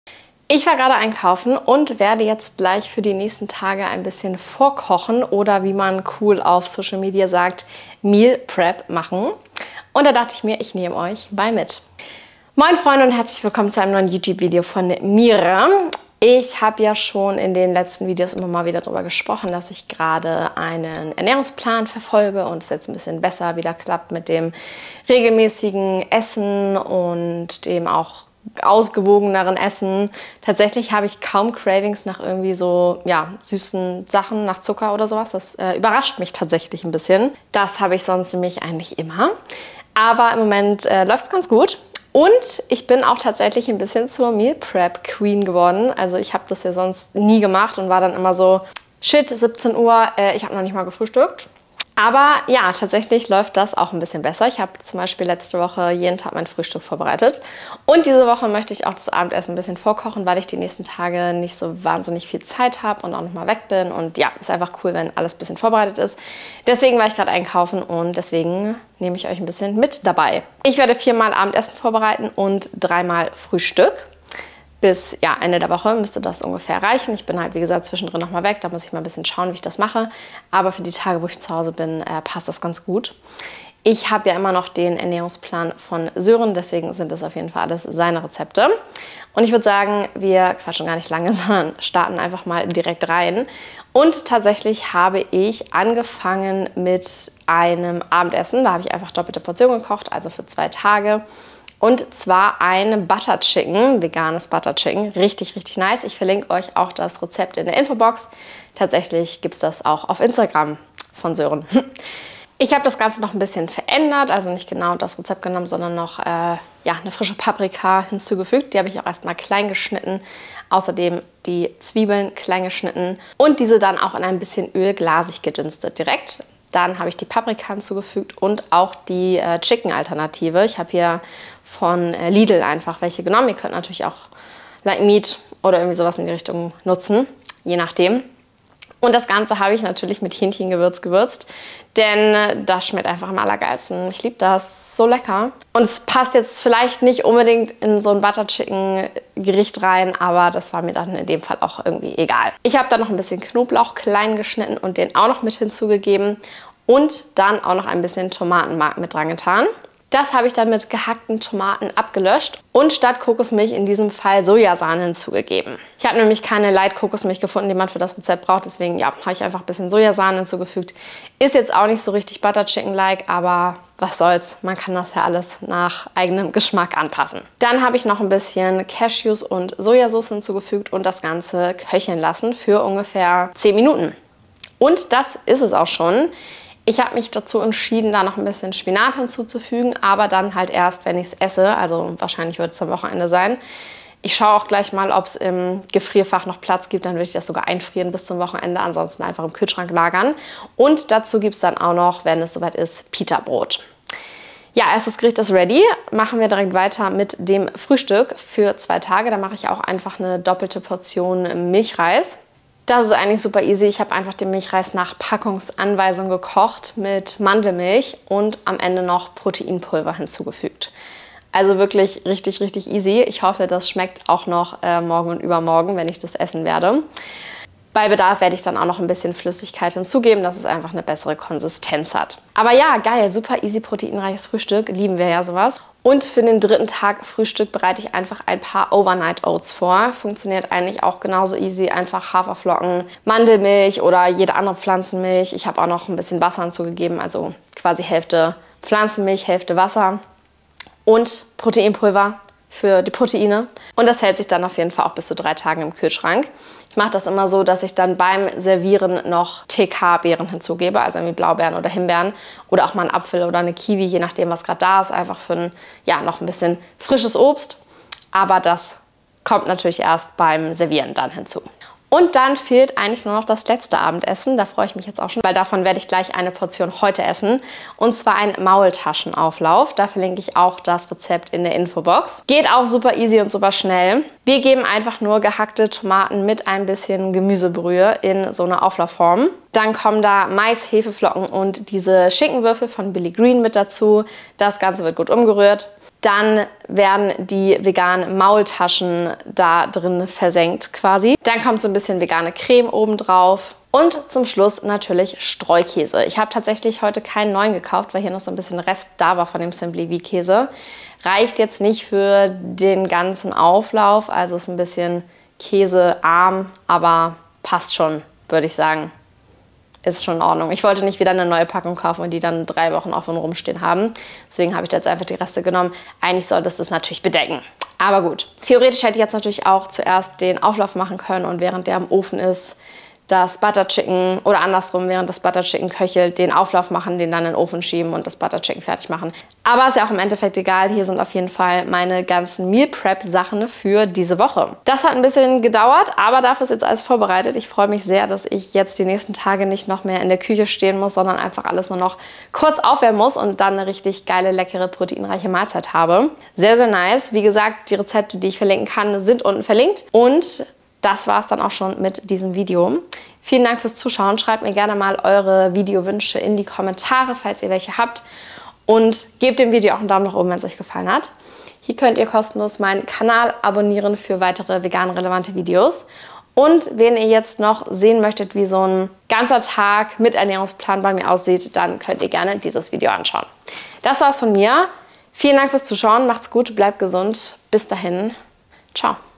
Meal Prep vegan & HIGH PROTEIN | 4 Rezepte vorkochen
Ich habe mich heute dazu entschieden, Queen of Meal Prep zu werden - vegan und high protein natürlich, nach meinem aktuellen Ernährungsplan. 🥑 IIch habe 4 Rezepte vorgekocht, 2x Frühstück und 2x Abendessen.